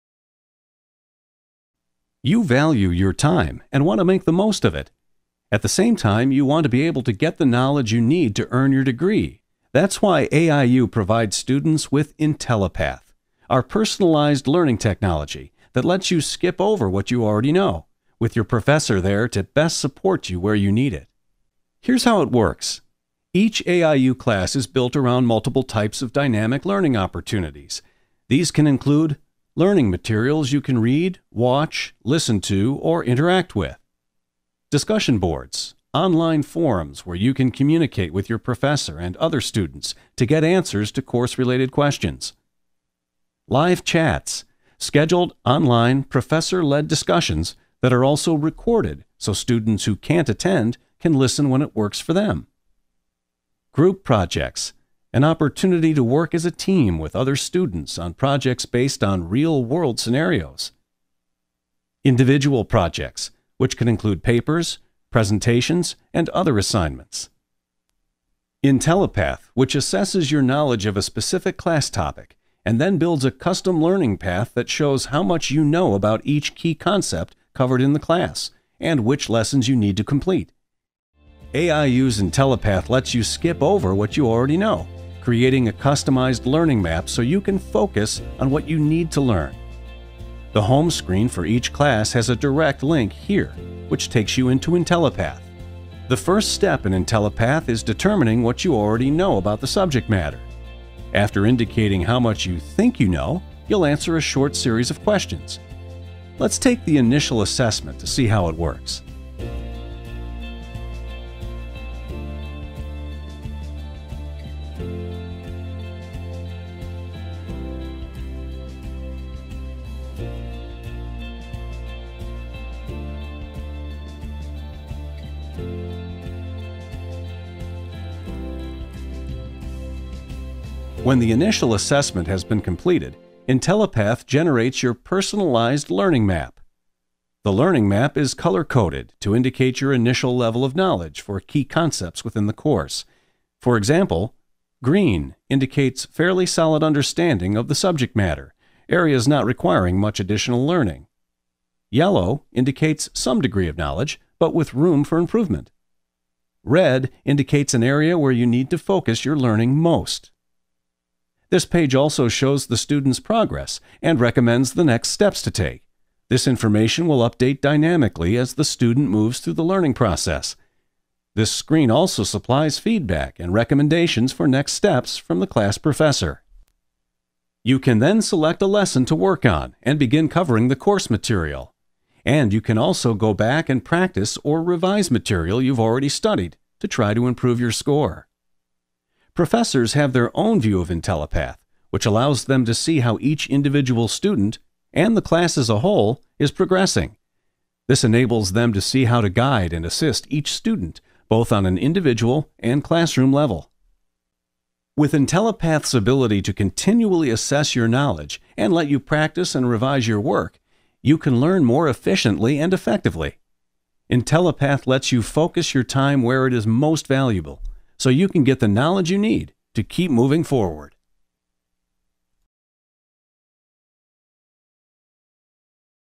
AIU intellipath®: Working in intellipath
AIU’s intellipath is an online learning tool that helps you build a curriculum for your classes based on what you already know. Using a series of questions related to the course, intellipath customizes your coursework to focus on providing content based on what you still need to learn.